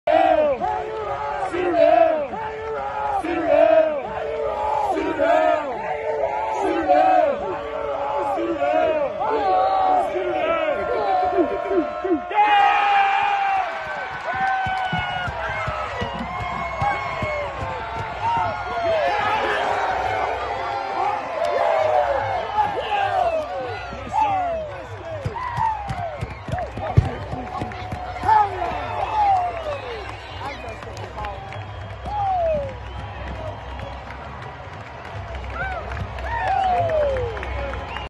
EAST CAROLINA 4 VIRGINIA 2 | FINAL OUT!
The Pirates get the final out to beat Virginia in the 2022 Greenville Regional winner's bracket game.

video credit: @ecujungle / Jared Plummer